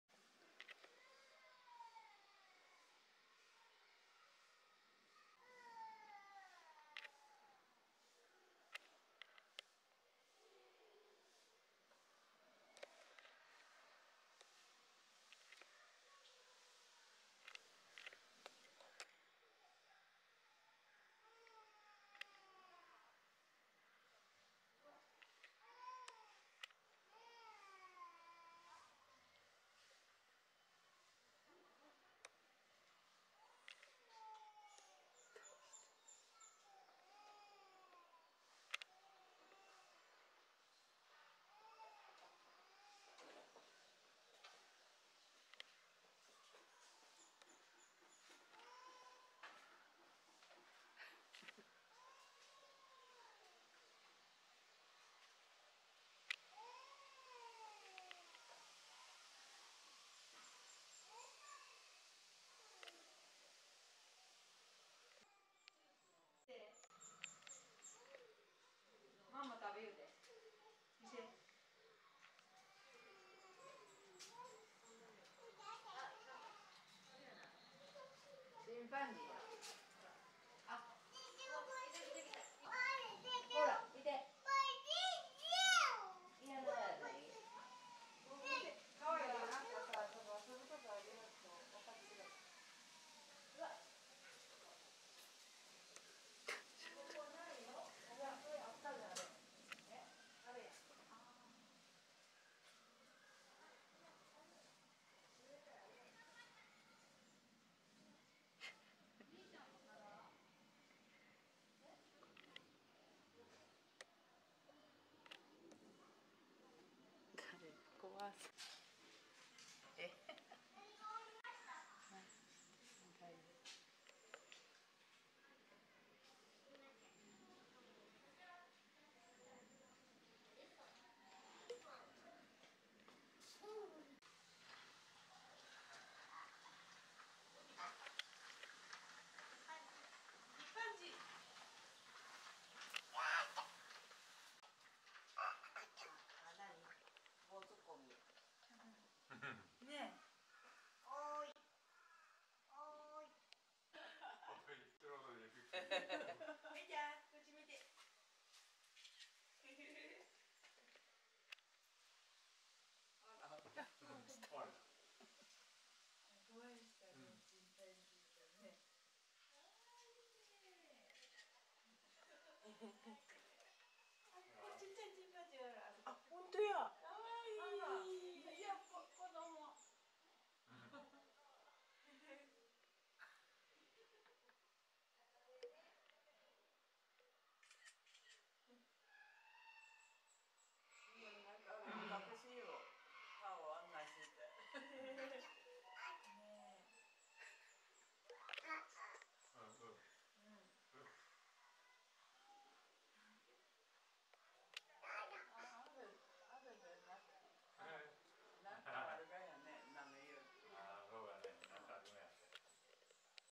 【Daiya♂】Chimpanzee Encounters Bucket: Episode2 チンパンジー、バケツとの遭遇:第2話
Enjoy the entire process of chimpanzees encountering the bucket!

Daiya♂
Taken：May 2024／Noichi Zoo Logical Park of Kochi Pref.
Please accept my apologies if there were any mistakes or inaccuracies. English is not my native language, so there may be some mistakes.)

バケツとの遭遇、一部始終をお楽しみください！

ダイヤ♂
撮影：2024年5月／高知立のいち動物公園